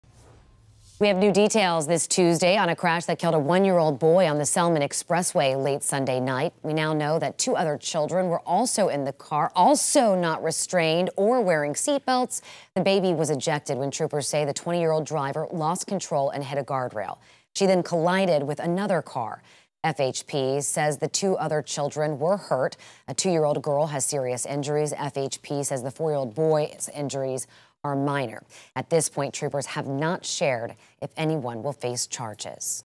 FHP: 1-year-old killed, 2 others hurt in crash on Selmon Expressway
The 1-year-old reportedly wasn't restrained in the car at the time of the crash. The driver of the car was seriously hurt.